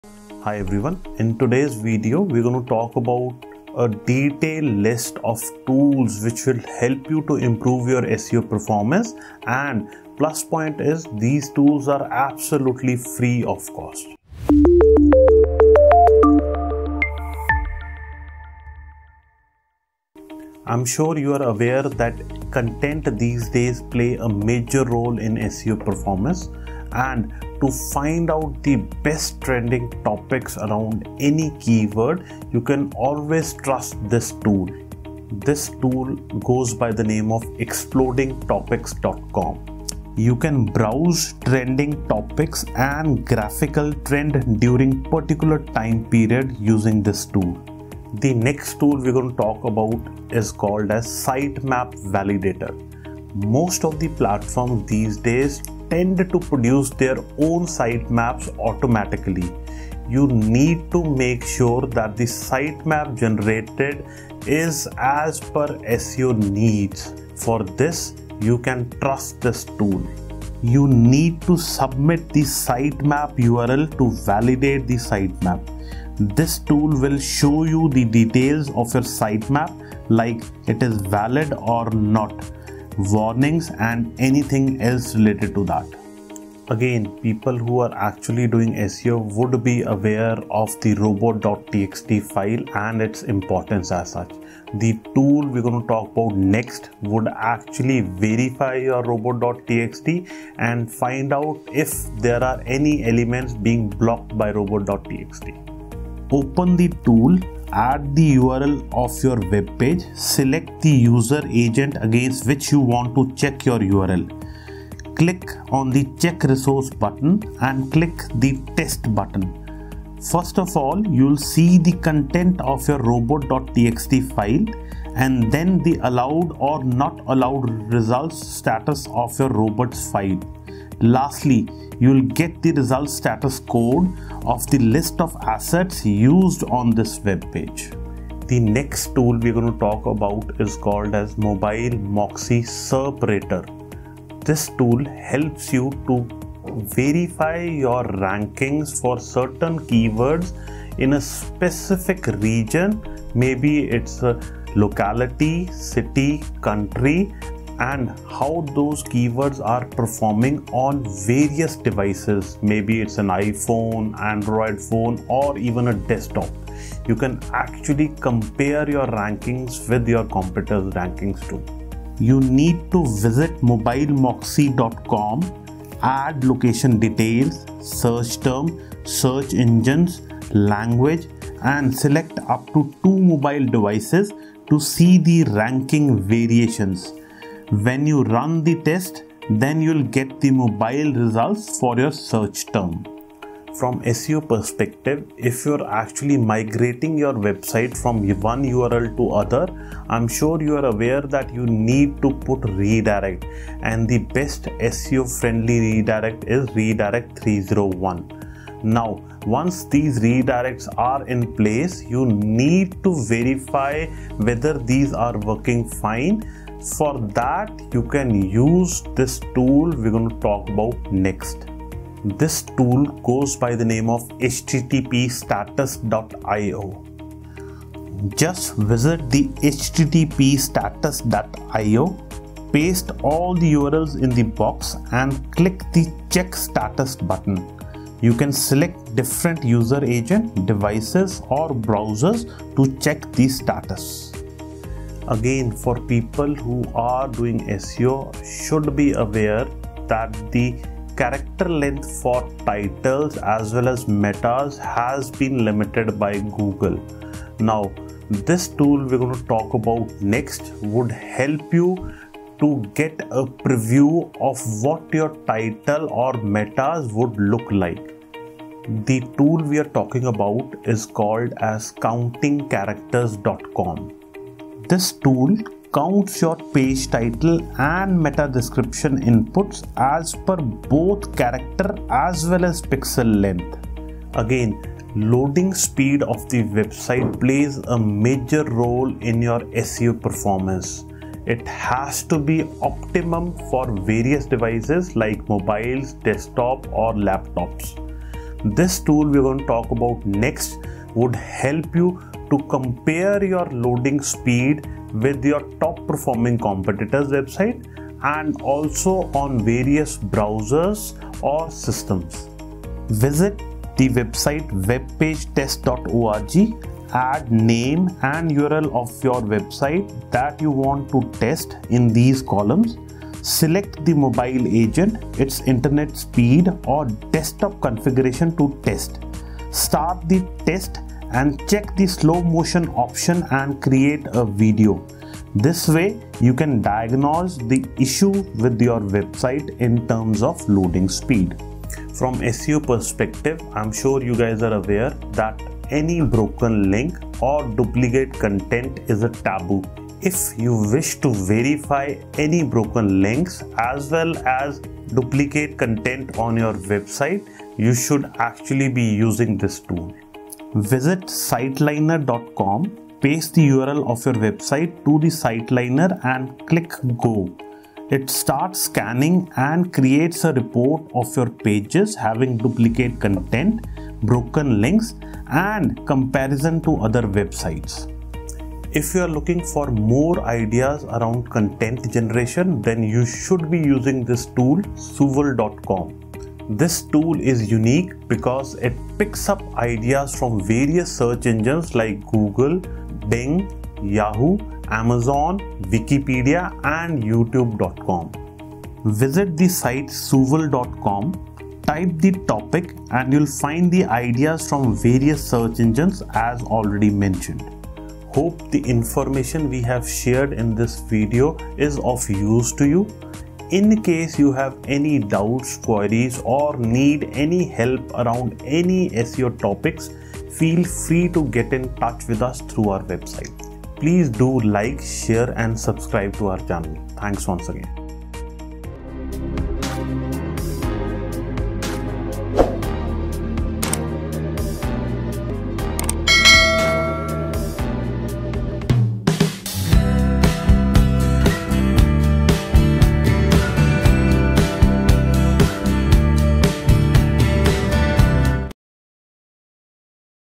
FREE SEO Tools to Boost Website Ranking 2024 | Free SEO Analysis Tools
Don't Waste Time & Money on SEO Tools! Use FREESEOanalysisTools to increase your chances of success. Need Help?

Hi everyone in today's video we're going to talk about a detailed list of tools which will help you to improve your SEO performance and plus point is these tools are absolutely free of cost.

I'm sure you are aware that content these days play a major role in SEO performance and to find out the best trending topics around any keyword you can always trust this tool.

This tool goes by the name of explodingtopics.com you can browse trending topics and graphical Trend during particular time period using this tool. The next tool we're going to talk about is called a site map validator, most of the platform these days tend to produce their own site Maps automatically you need to make sure that the site map generated is as per SEO needs for this you can trust this tool. You need to submit the sitemap URL to validate the sitemap this tool will show you the details of your site map like it is valid or not, warnings and anything else related to that.

Again people who are actually doing SEO would be aware of the robots.txt file and its importance as such the tool we're going to talk about next would actually verify your robot.txt and find out if there are any elements being blocked by robot.txt.

The next tool we're going to talk about is called as mobile Moxie separator this tool helps you to verify your rankings for certain keywords in a specific region maybe it's a locality, City, Country and how those keywords are performing on various devices.

From SEO perspective if you are actually migrating your website from your one URL to other I am sure you are aware that you need to put redirect and the best SEO friendly redirect is redirect 301 now once these redirects are in place you need to verify whether these are working fine for that you can use this tool we're going to talk about next this tool goes by the name of HTTPstatus.io.

Again for people who are doing SEO should be aware that the character length for titles as well as Metals has been limited by Google Now this tool we're going to talk about next would help you to get a preview of what your title or metas would look like the tool we are talking about is called as Counting characters.com

This tool counts your page title and meta description inputs as per both character as well as pixel length

Again loading speed of the website plays a major role in your SEO performance it has to be Optimum for various devices like mobiles desktop or laptops this tool we're going to talk about next would help you to compare your loading speed with your top performing competitors website and also on various browsers or systems. Visit the website webpagetest.org add name and URL of your website that you want to test in these columns select the mobile agent its internet speed or desktop configuration to test start the test and check the slow motion option and create a video this way you can diagnose the issue with your website in terms of loading speed.

From SEO perspective I'm sure you guys are aware that any broken link or duplicate content is a taboo if you wish to verify any broken links as well as duplicate content on your website you should actually be using this tool, visit siteliner.com paste the URL of your website to the site liner and click go it starts scanning and creates a report of your pages having duplicate content broken links and comparison to other websites.

If you are looking for more ideas around content generation then you should be using this tool soovle.com this tool is unique because it picks up ideas from various search engines like Google Bing Yahoo Amazon Wikipedia and youtube.com visit the site soovle.com type the topic and you'll find the ideas from various search engines as already mentioned.

Hope the information we have shared in this video is of use to you in case you have any doubts queries or need any help around any SEO topics feel free to get in touch with us through our website please do like share and subscribe to our Channel thanks once again foreign